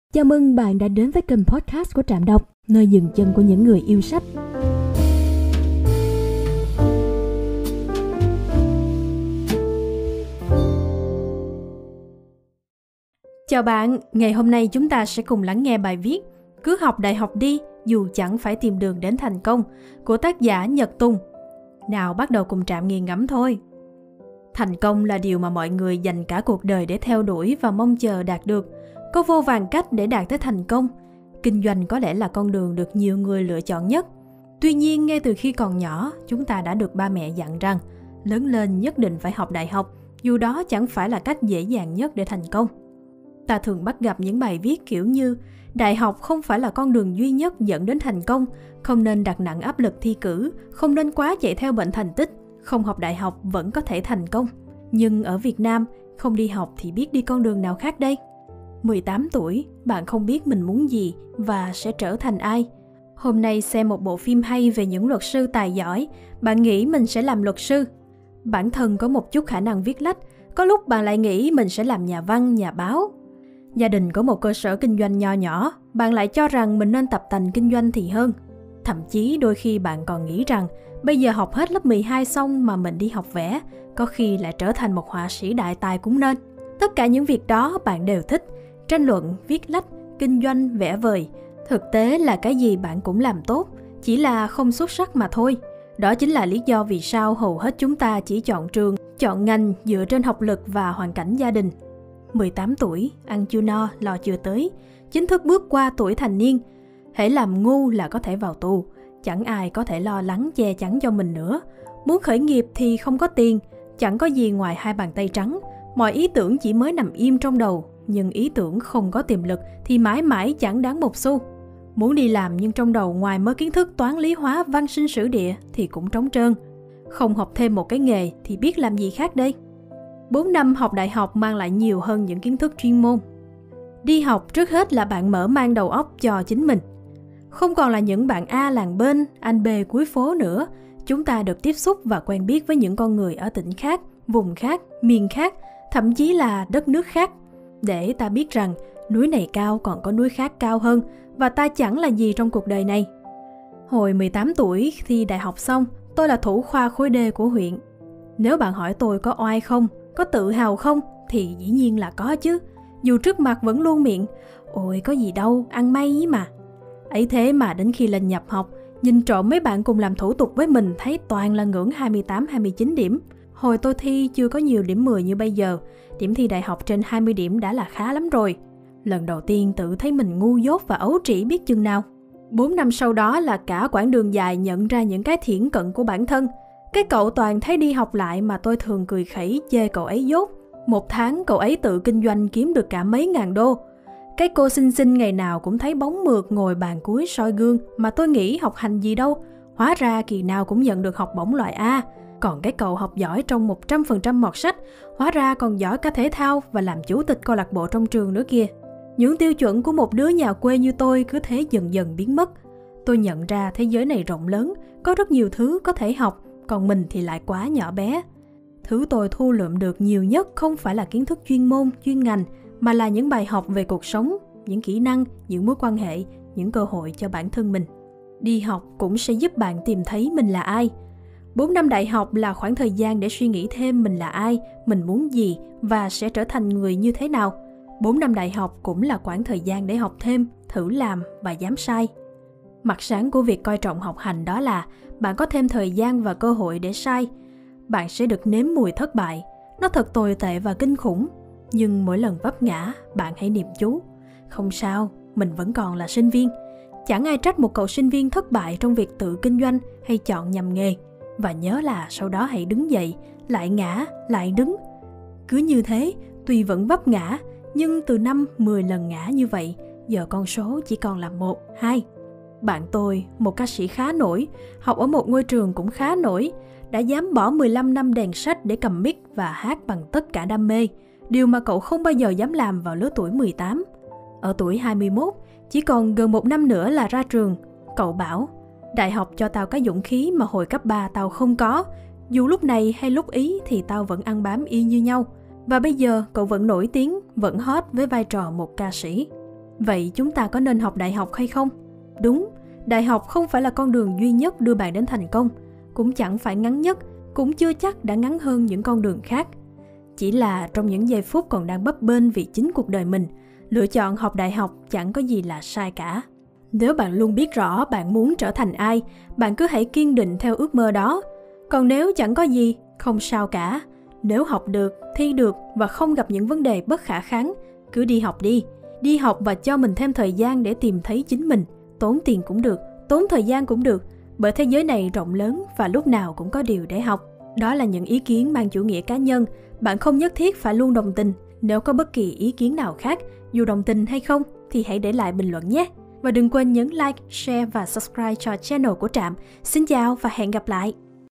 Mục đích của cuộc đời là một cuộc đời có mục đích | Trạm Nghiền Ngẫm | Trạm Đọc
Theo bạn, mục đích của cuộc đời có phải là 1 điều quan trọng không? Có người cho rằng chỉ khi hiểu rõ về con đường bạn đang bước đi, bạn mới có được một tâm hồn toàn vẹn và tĩnh lặng.  "Robert Bryne từng nhận thấy, “Mục đích của cuộc đời chính là một cuộc đời có mục đích."" Để đạt được điều gì, bạn cần vạch rõ mục tiêu cuối cùng. Điều này rất quan trọng. Và một khi xác định những điều cần làm sớm, mọi thứ sẽ trở nên rõ ràng hơn. Một cuộc sống không có mục đích chính là một cuộc sống không có điểm đến.  Tìm được hướng đi đúng cho cuộc sống là một vấn đề thực tại với tất cả chúng ta. Bạn mong đợi điều gì trong cuộc sống? Phải sống mà không biết bạn muốn gì có thể rất nguy hiểm."